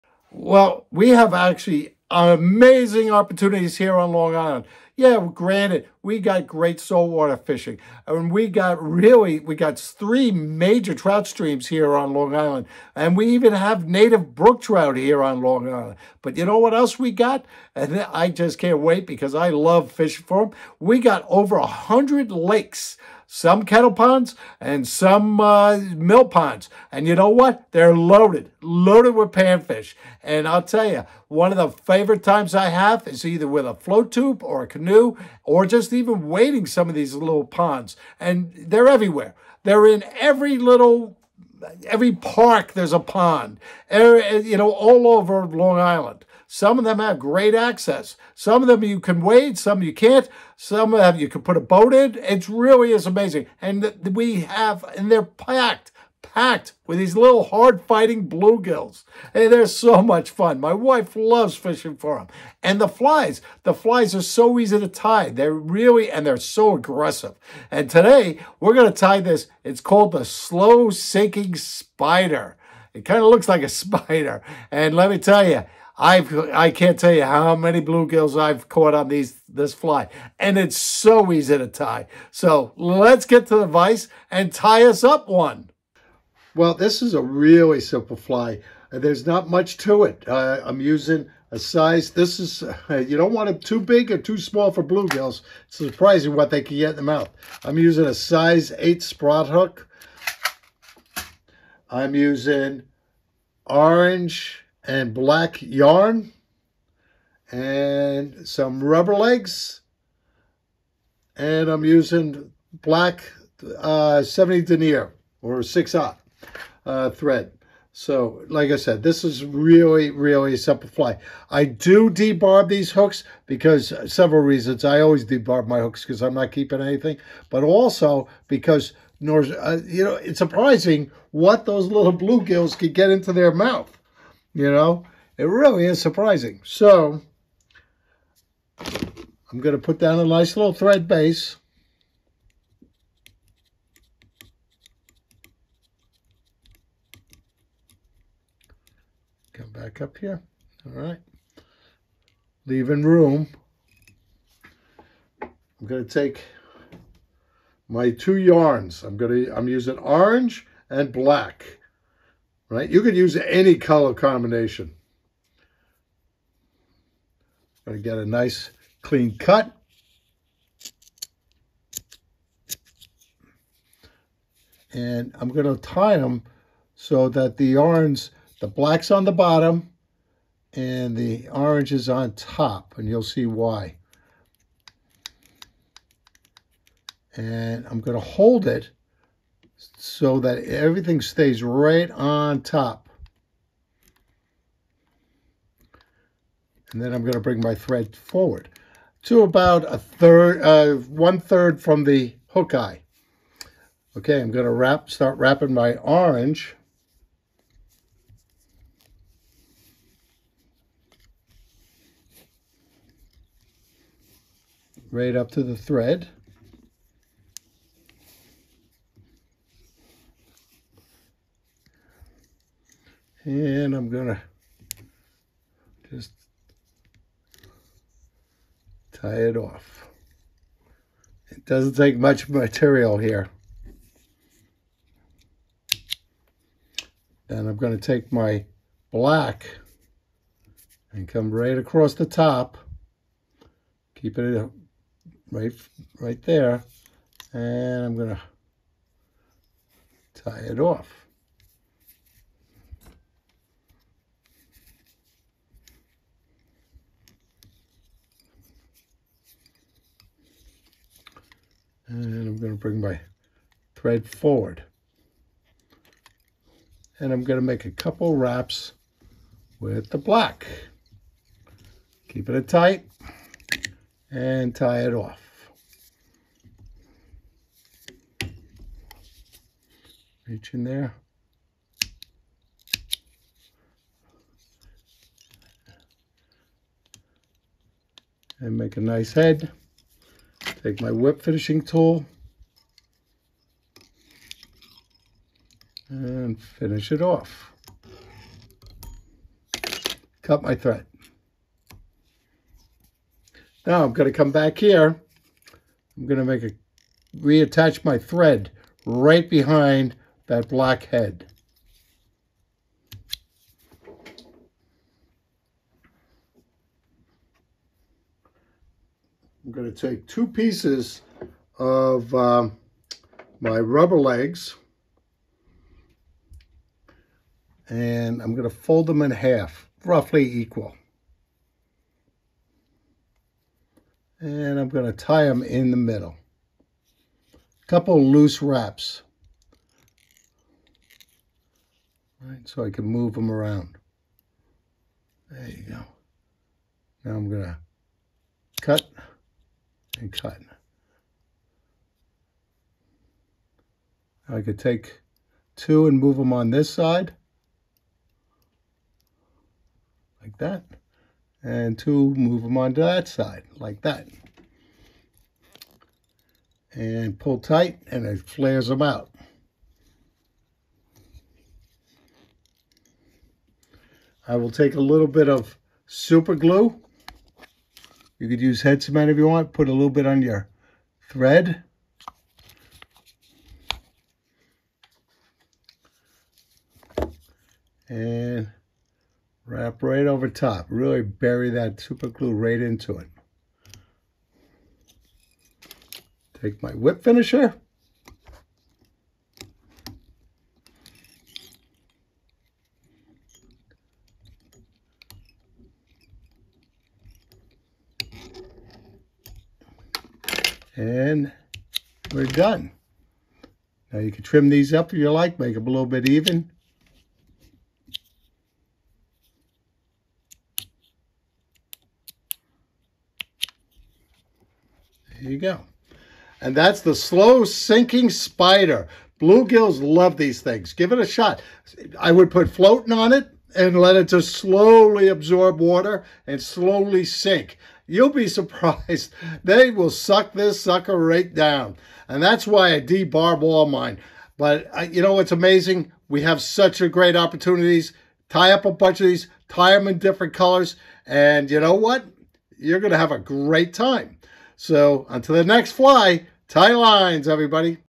How to tie the Slow Sinking Spider
Paul McCain of River Bay Outfitters show us how to tie the Slow Sinking Fly Spider